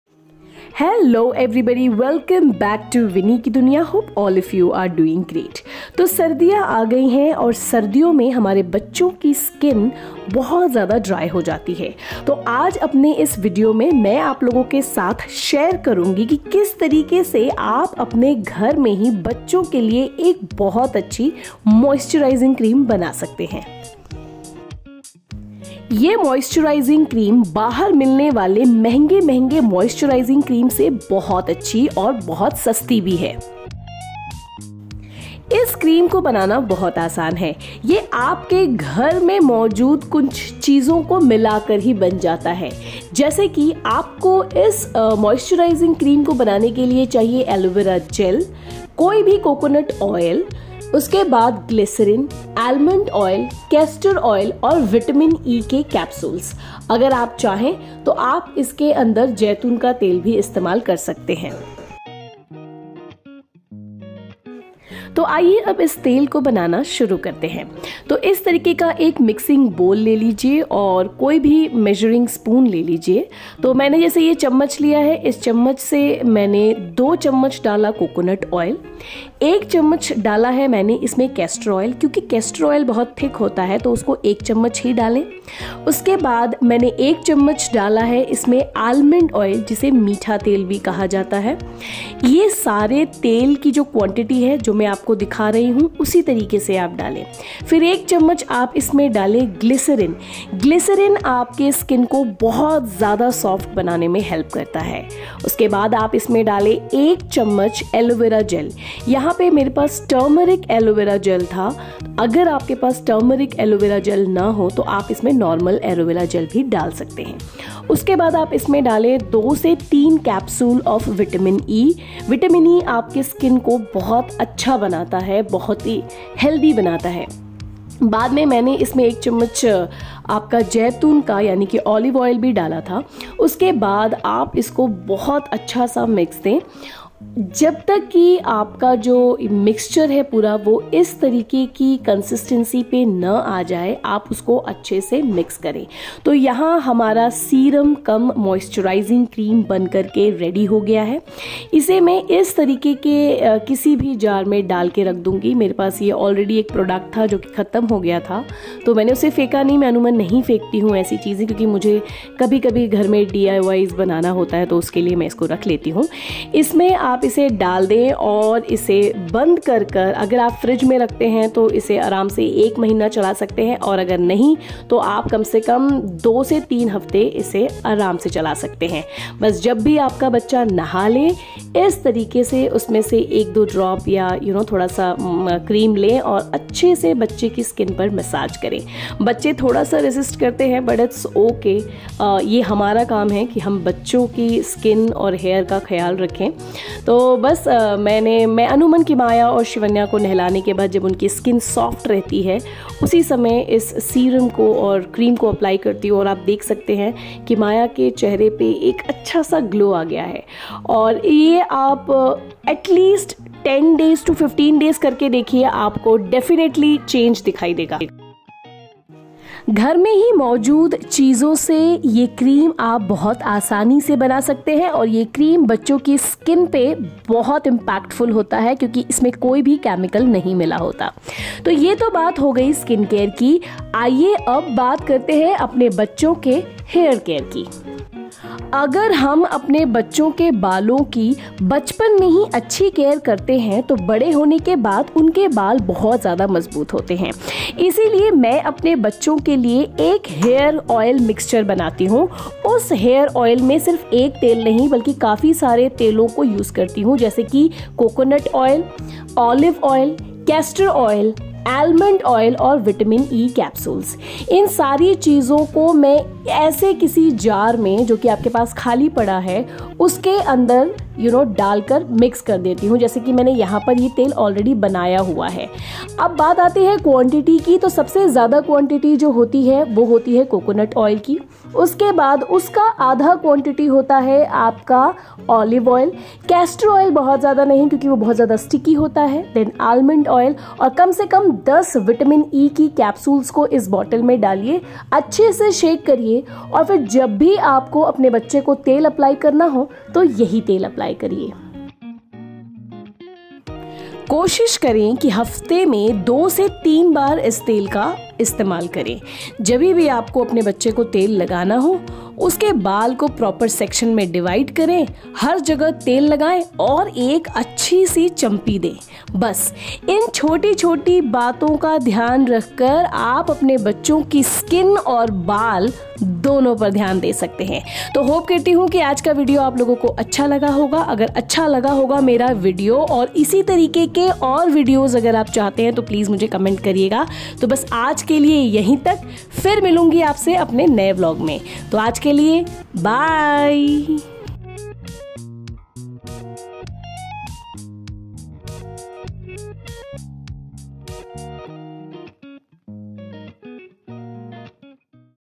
Homemade moisturizing cream and hair oil for kids@vinikiduniya9267
Hello Everybody,
The skin of our kids are very sensitive so, we have to take care of that specially in winters because winters make their skin even more dry and flaky, that's why in today's video i am going to share with you all the DIY for homemade moisturizing cream and hair oil.